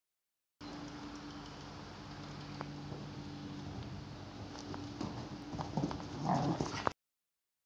Welsh Pony Free Jumping
Section B Welsh pony mare Rosie working through a jump chute to 2'6".